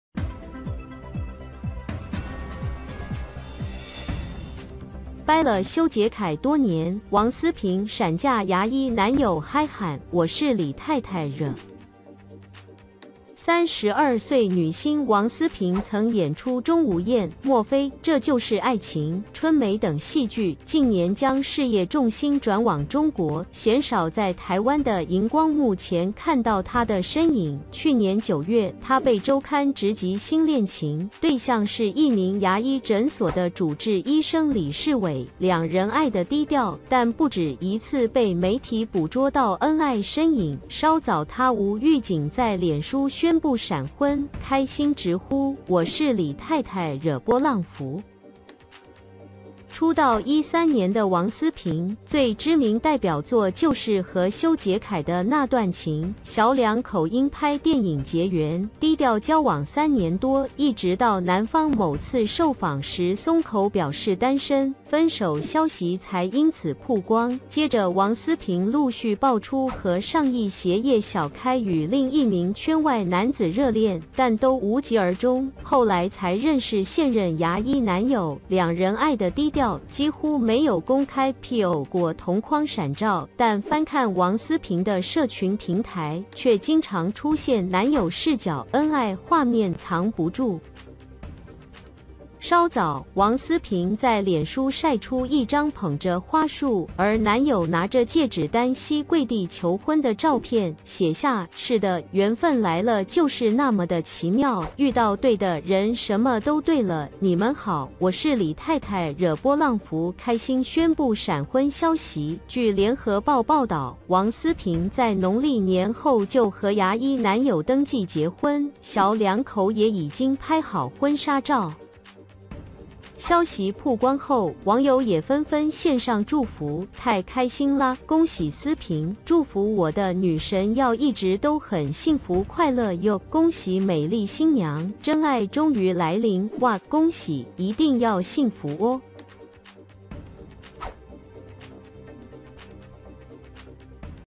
掰了修杰楷多年！王思平閃嫁牙醫男友 嗨喊：我是李太太惹:更新新闻
娛樂中心／李嘉翎報導

32歲女星王思平曾演出《鍾無艷》、《莫非，這就是愛情》、《春梅》等戲劇，近年將事業重心轉往中國，鮮少在台灣的螢光幕前看到她的身影。去年9月，她被週刊直擊新戀情，對象是一名牙醫診所的主治醫生李世偉，2人愛的低調，但不只一次被媒體捕捉到恩愛身影，稍早她無預警在臉書宣布閃婚，開心直呼：「我是李太太惹～」